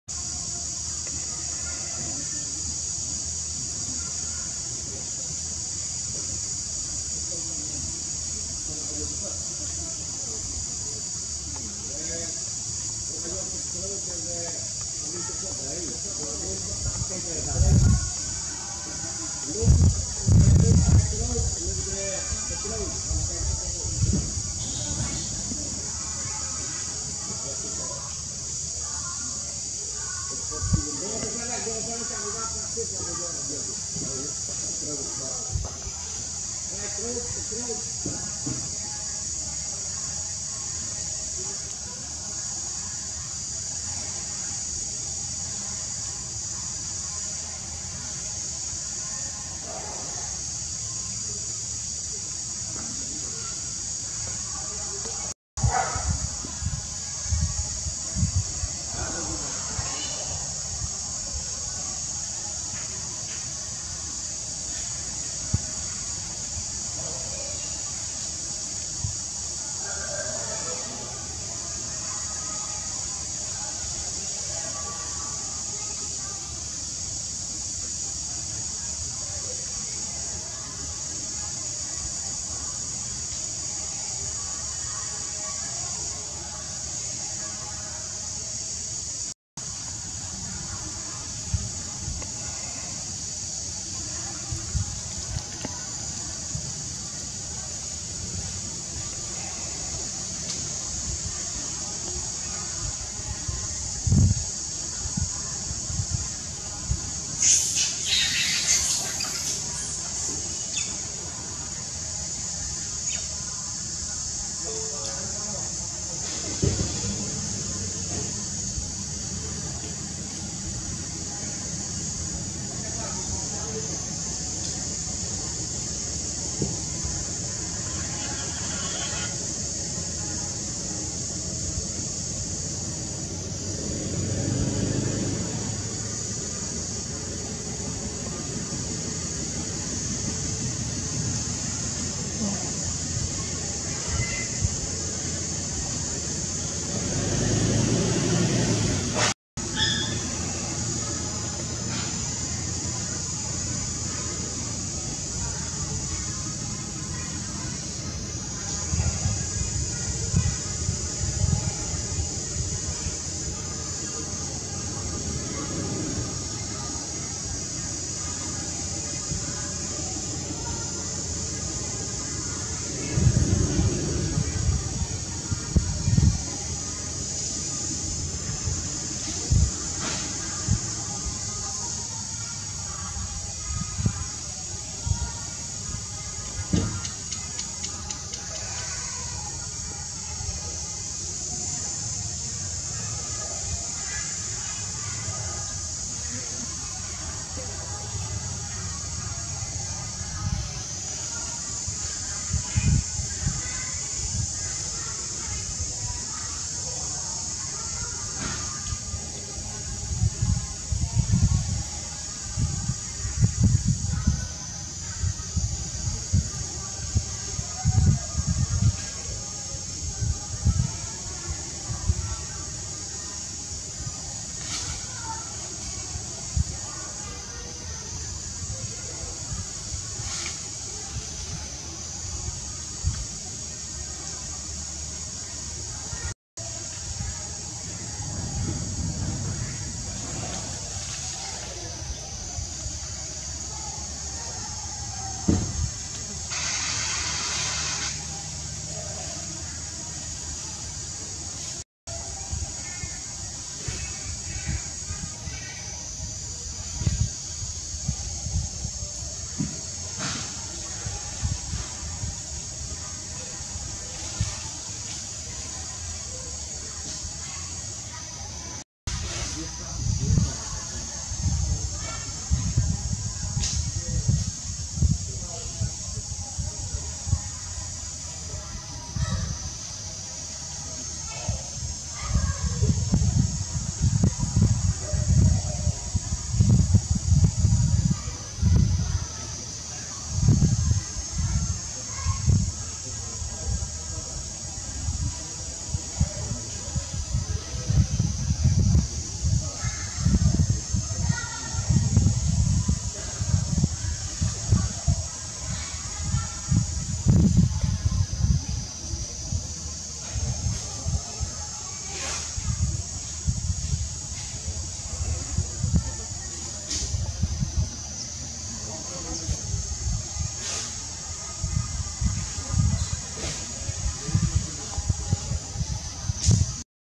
These animals are very scared, and they hate the noise and the smell
Dogs never eat meat as food, but it can bite our children, let's not let it close, it gets faster.